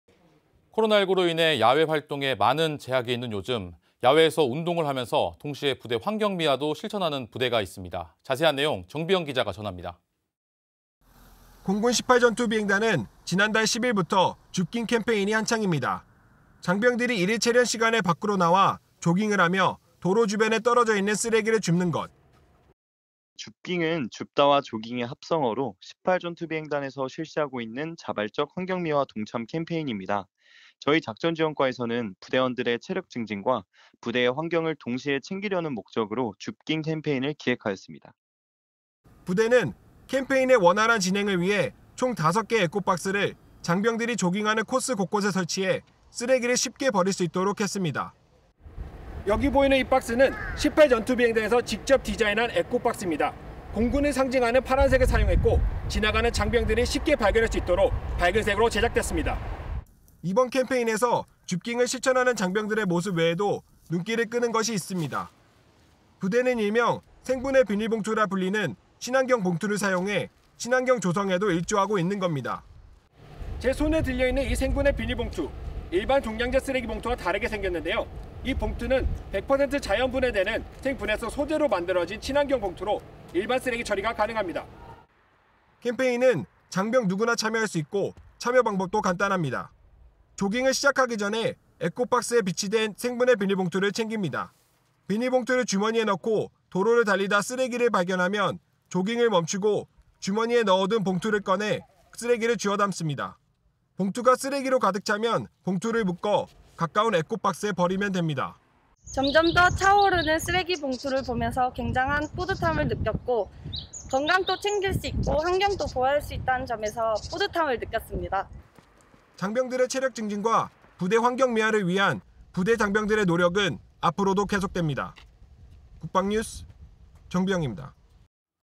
[공군 18전비, ‘줍깅 캠페인’ ] '운동도 하고 쓰레기도 줍고' 공군 18비의 '일타쌍피'
공군 18전비, ‘줍깅 캠페인’ 전개

코로나19로 인해 야외활동에 많은 제약이 있는 요즘. 야외에서 운동을 하면서 동시에 부대 환경미화도 실천하는 장병들이 있어 눈길을 끌고 있습니다. 자세한 내용, 정비형 기자가 전해드립니다.

공군 18전투비행단은 지난 달 10일부터‘줍깅 캠페인’이 한창입니다.

장병들이 일일체련시간에 밖으로 나와 조깅을 하며 도로주변에 떨어져있는 쓰레기를
줍는 것.

부대는 캠페인의 원활한 진행을 위해 총 다섯 개의 ‘에코박스’를 장병들이 조깅하는
코스 곳곳에 설치해 쓰레기를 쉽게 버릴 수 있도록 했습니다.

여기 보이는 이 박스는 18전투비행단에서 직접 디자인한 ‘에코박스’
입니다. 공군을 상징하는 파란색이 사용됐고 지나가는 장병들이 쉽게 발견할 수
있도록 밝은 색으로 제작됐습니다.

이번 캠페인에서 줍깅을 실천하는 장병들의 모습 외에도 눈길을 끄는 것이 있습니다.

부대는 일명 ‘생분해 비닐봉투’라 불리는 친환경 봉투를 사용해 친환경 조성에도
일조하고 있는 겁니다.

제 손에 들려있는 이 ‘생분해 비닐봉투.’
일반 종량제쓰레기봉투와 다르게 생겼는데요. 이 봉투는 100퍼센트 자연 분해되는
생분해성 소재로 만들어진 친환경 봉투로 일반쓰레기 처리가 가능합니다.

캠페인은 장병 누구나 참여할 수 있고 참여방법도 간단합니다.

조깅을 시작하기 전에 ‘에코박스’안에 비치된 ‘생분해 비닐봉투’를 챙깁니다.
비닐봉투를 주머니에 넣고 도로를 달리다 쓰레기를 발견하면 조깅을 멈추고
주머니에 넣어둔 봉투를 꺼내 쓰레기를 주워 담습니다.
봉투가 쓰레기로 가득 차면 봉투를 묶어 가까운 ‘에코박스’에 버리면 됩니다.

장병들의 체력증진과 부대 환경미화를 위한 부대 장병들의 노력은 앞으로도
계속됩니다.

국방뉴스 정비형입니다.